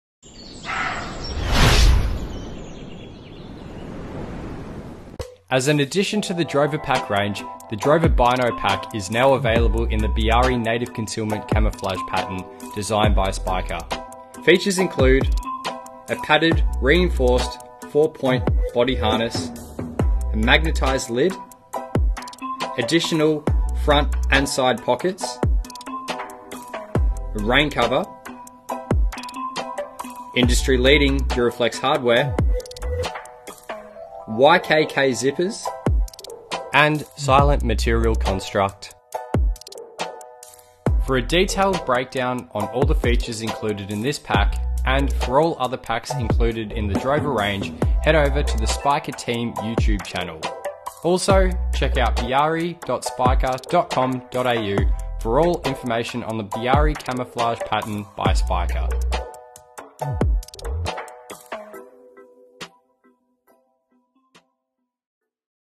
SPIKA Camo Hunting Chest Pack
◀ SPIKA Hunting Chest Pack ▶
📦 VIEW ON AMAZON
Optimizing comfort, accessibility, and functionality the drover bino harness is for serious spot and stalk hunters. The main compartment protects your optics while the magnetized lid secures your gear whatever your hunting style. Durable 500 denier DWR treated nylon body and reinforced base Duraflex hardware lightweight makes the bag more comfortable to use.
🏷️ CHECK THE PRICE ON AMAZON